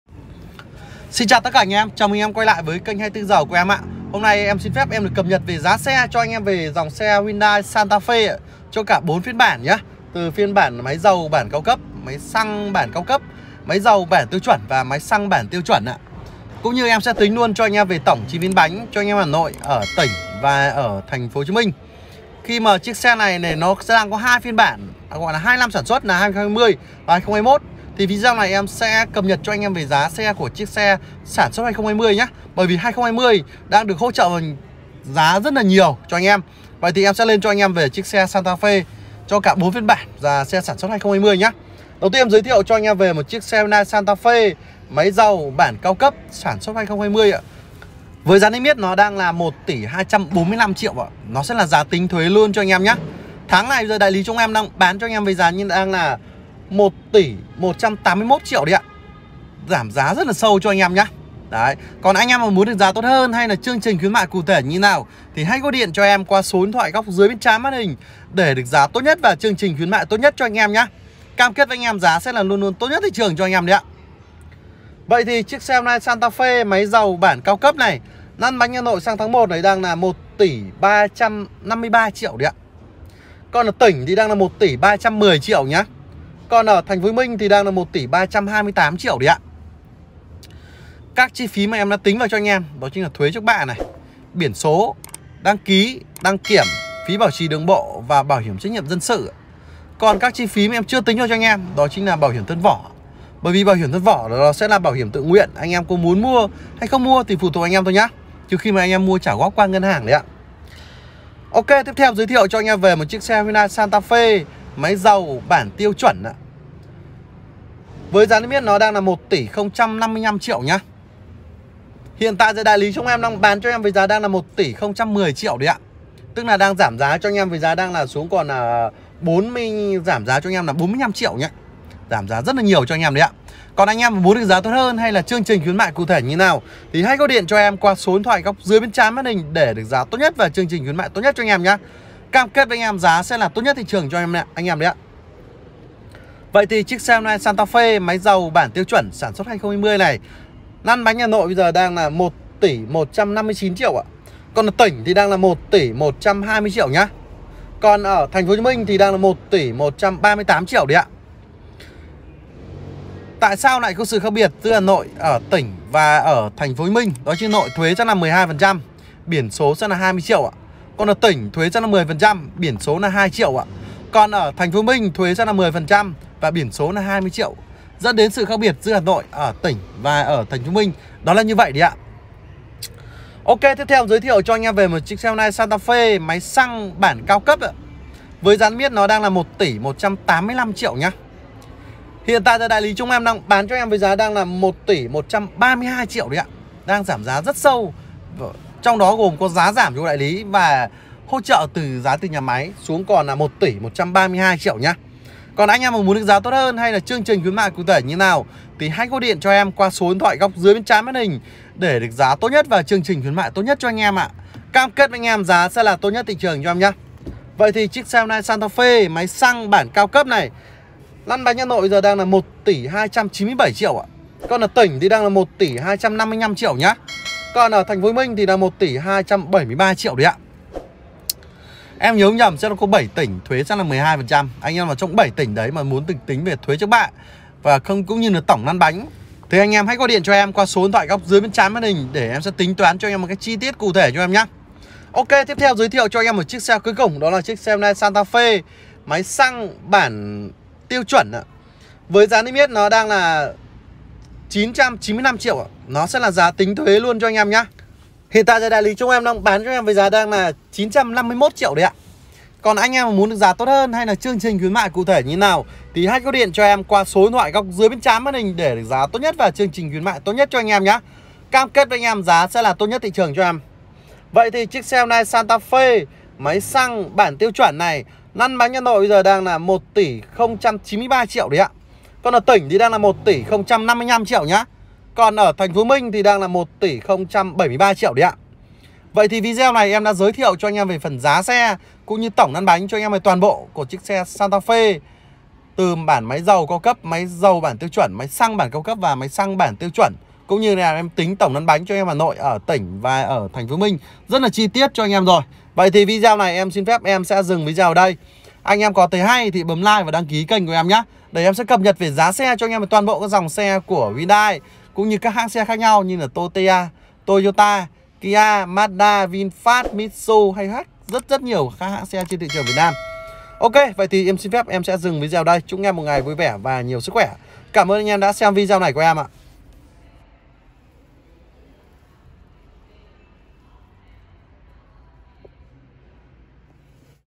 Tổng lăn bánh Hyundai Santafe 2021, giá xe tặng 50% thuế TB
- Giá xe Hyundai Santafe 2021 máy dầu cao cấp - 1.221tr
- Giá xe Hyundai Santafe 2021 máy dầu tiêu chuẩn -  1.040tr
- Giá xe Hyundai Santafe 2021máy xăng cao cấp - 1.162tr
- Giá xe Hyundai Santafe 2021 máy xăng tiêu chuẩn -  981tr
* Tổng lăn bánh Hyundai Santafe 2021, giá xe tặng 50% thuế TB thì hãy click vào đường link nhé.
* Các màu xe của Hyundai Santafe 2021:
 - Màu đen (NKA)
 - Màu trắng (PW6)
 - Màu Bạc (T8S)
 - Màu Vàng cát (S8N)
 - Màu xanh đại dương (ST2)
 - Màu đỏ đô (S5W)
Tổng lăn bánh Hyundai Santafe 2021, giá xe tặng 50% thuế TB các dòng xe khác của Hyundai: